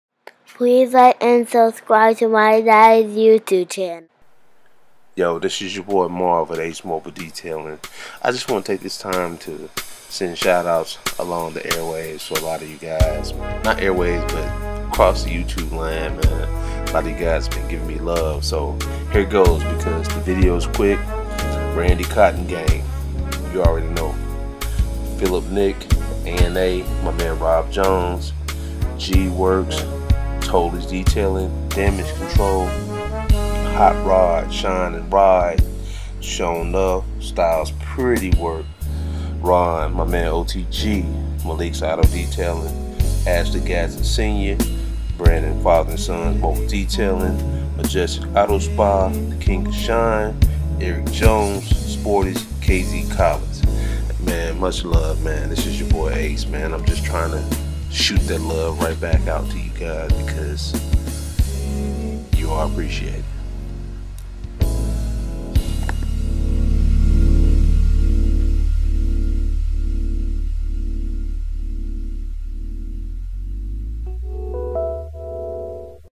Shout Outs
I'm at 50 subscribers and want to take the time to send some shout outs to all of those who took the time to comment on my videos.